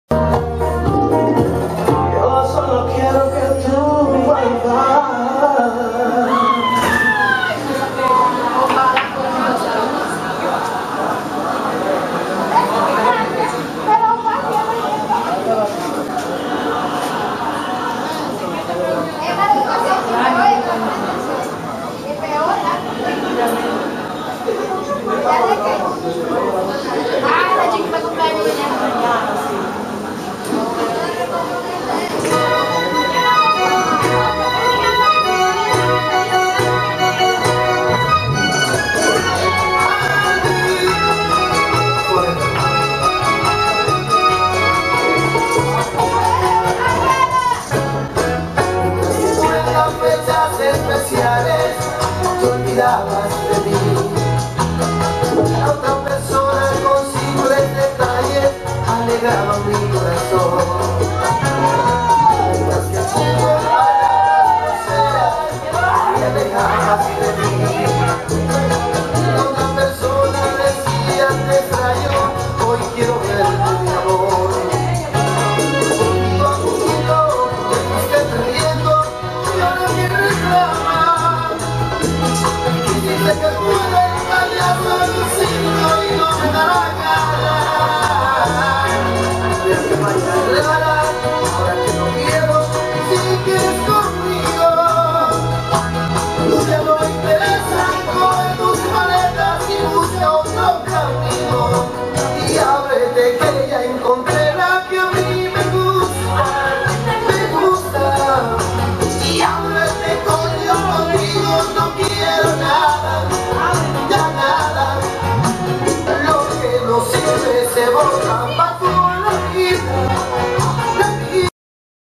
hanging with my dad @ luquillo beach pt1
went to luquillo beach wih my dad and lydia, we ate food from the kioskos, had a few beers, the we chilled by the water, then we hit the pool table